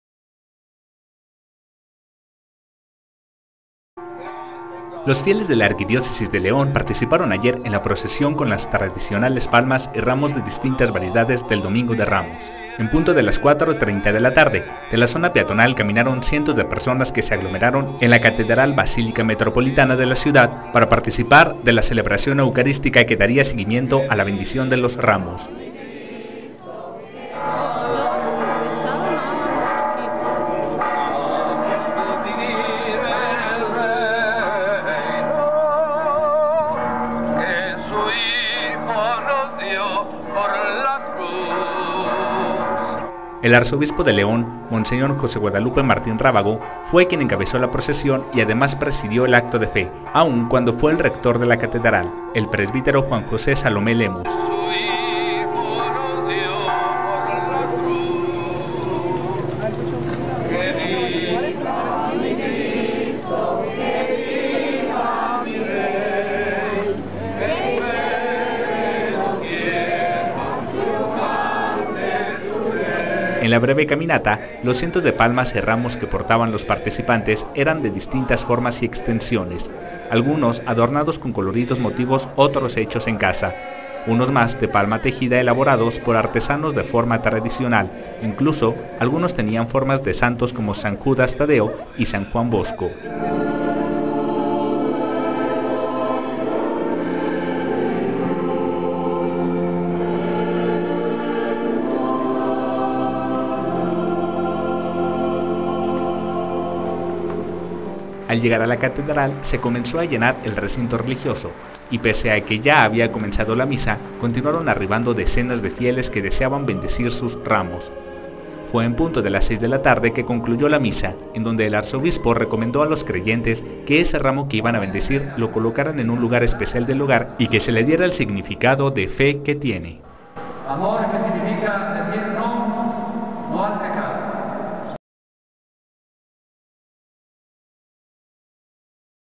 Bendicen las palmas y los ramos
Encabeza Arzobispo tradicional procesión de palmas en la Zona Peatonal y misa en la Catedral Basílica.
Los fieles de la Arquidiócesis de León participaron ayer en la procesión con las tradicionales palmas y ramos de distintas variedades del Domingo de Ramos.